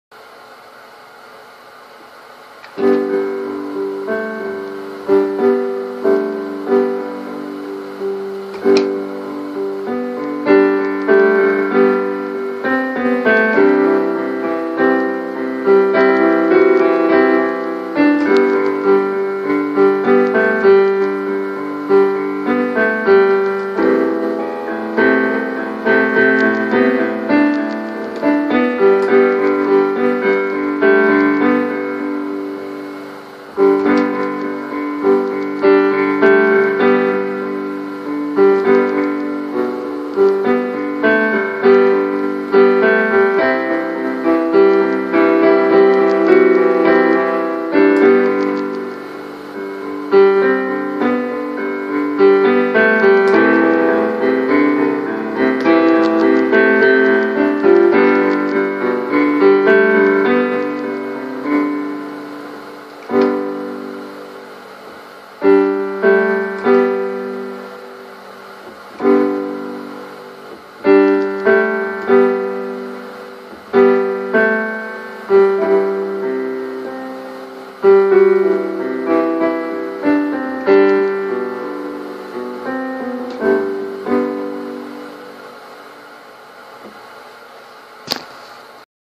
Piano by Lisa Jones, Original and Impromptu
Original Piano by Lisa Jones . I was feeling inspired and wanted to play without my head getting in the way, so I played impromptu. This music hear is original music by myself, Lisa Jones.
(Note: It is now recorded and published.)

My apologies for the roughness of it. Need a new piano pedal.